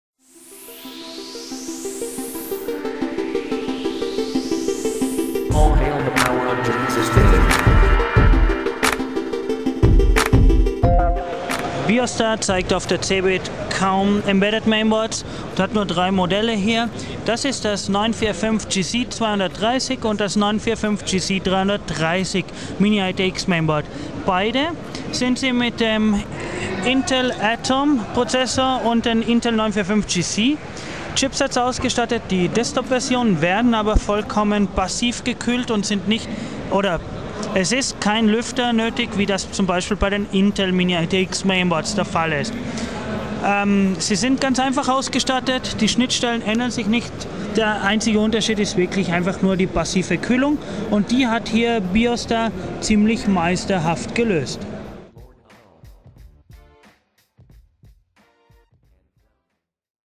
Biostar 945GC Mini-ITX Mainboard - CeBIT 2009
Erste Biostar Board mit Intel Atom 230 und Atom 330 Dual-Core CPU.